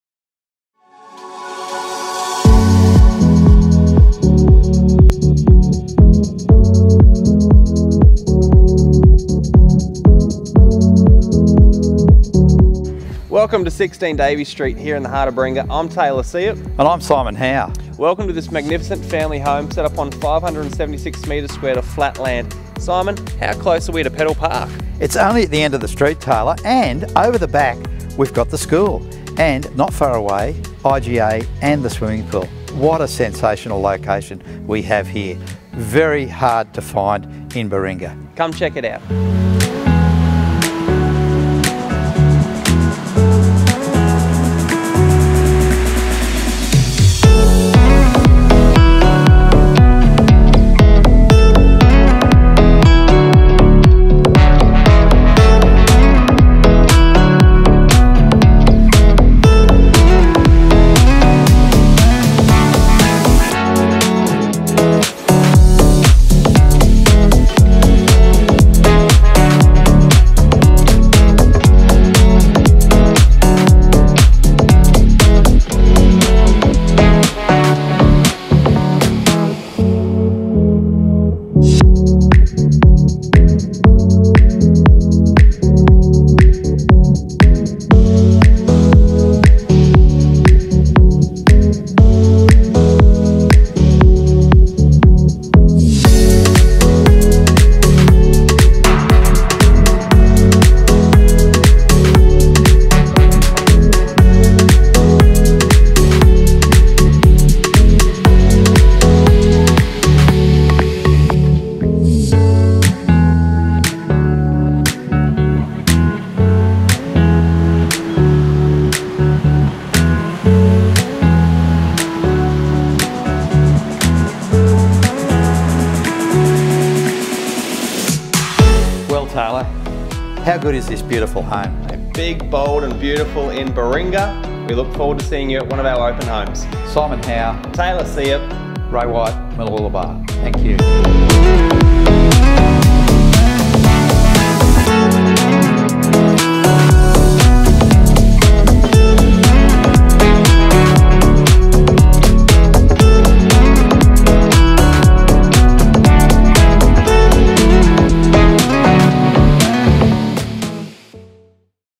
Big, Bold, Baringa Beauty
Welcome to 16 Davies St Baringa in the city of colour "Aura".

Set on a generous 576m2 level block you will find a magnificent family home with a cleverly designed floor plan to give the family room to move and spread out.

Convenience is a plus when buying here, we are walking distance to "Pedal Park" at the end of the street, local secondary school 500 mtrs & Primary school 900 mtrs.